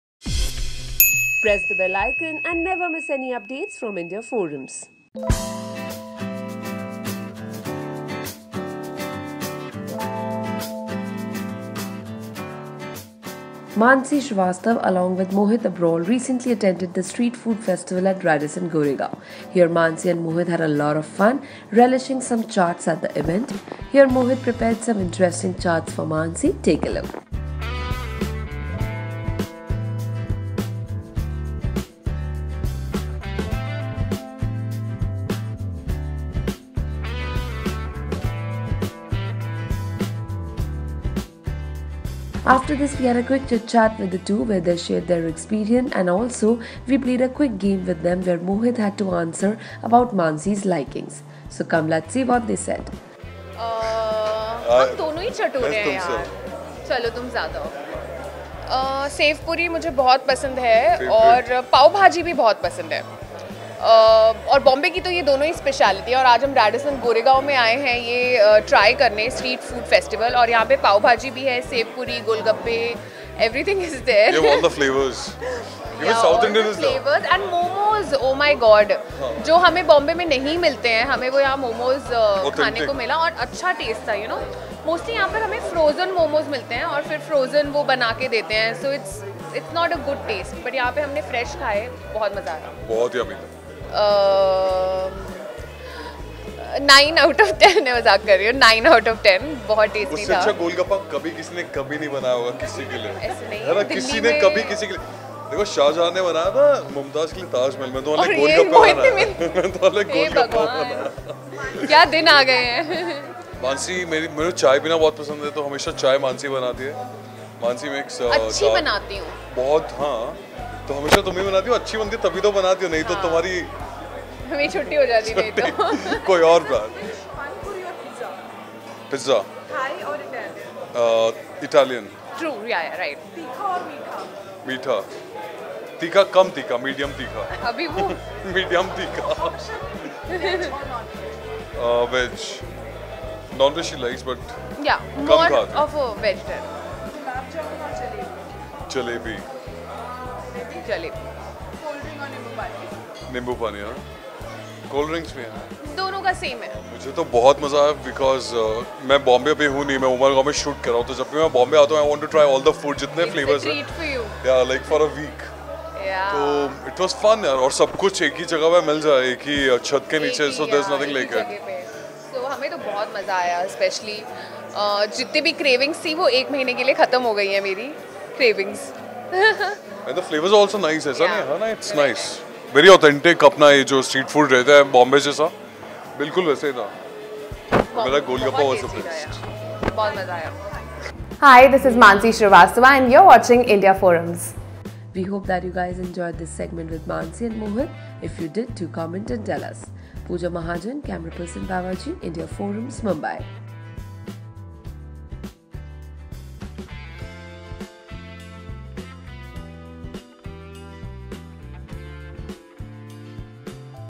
Mohit Abrol Makes Chaats For Mansi Srivastav | Exclusive Interview
Mansi Srivastav along with Mohit Abrol recently attended the street food festival at Radisson Goregaon. Here Mansi and Mohit had a lot of fun relishing some chaats at the event. Here Mohit prepared some interesting chaats for Mansi. After this we had a quick chat with the two where they shared their experience, and also we played a quick game with them where Mohit had to answer about Mansi’s likings We hope that you guys enjoyed this segment with Mansi and Mohit if you did do Comment and tell us.